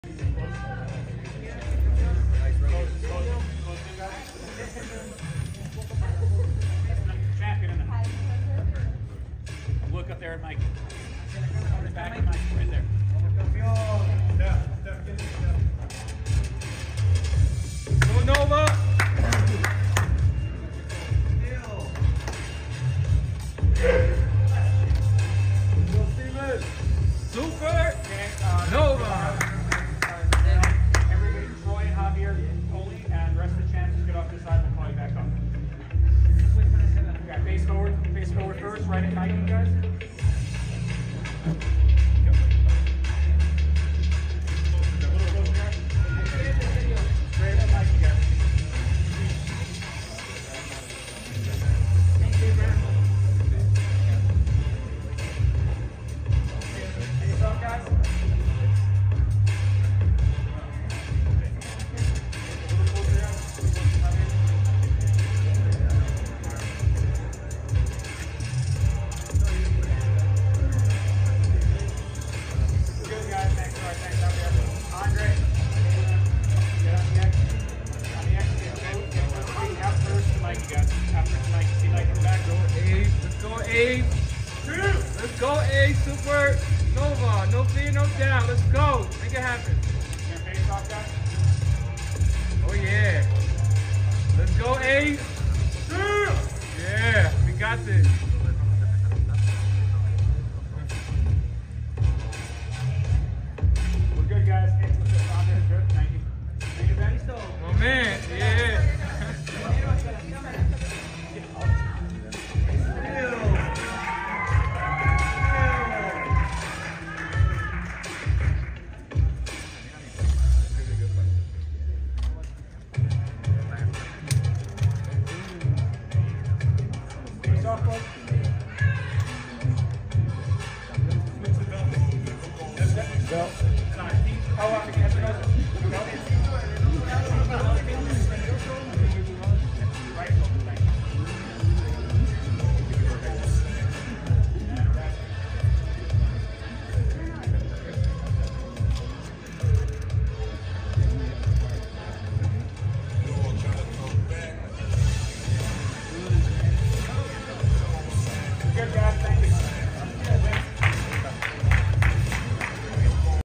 Abraham Nova & Andres Cortes TRADE WORDS & Rafael Espinoza STARES DOWN Sergio Chirino at FACE OFFS
FightHype's Ron Goodall was on hand in Las Vegas, Nevada where Rafael Espinoza faced off with Sergio Chirino and Andres Cortes took on Abraham Nova. You don't want to miss what went down before, during, and after the fights. Check it out.